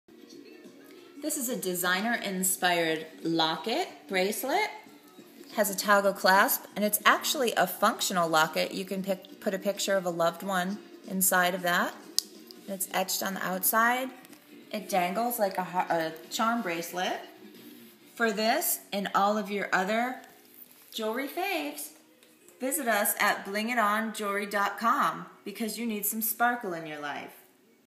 Locket Toggle Link Gold Tag Bracelet B483
Stoneless, Open Toggle Link Bracelet with Dangling Locket Tag. Set in 50 Mils of Heavily Electroplated 14K Yellow Gold over Jewelry Brass and is Secured by a Toggle Clasp. This Heavy Amount of Gold Plating will Never Chip, Turn or Tarnish, so the Piece will Last For a Lifetime. Keep a Photo of your Loved one in this Functional, Fashionable Piece.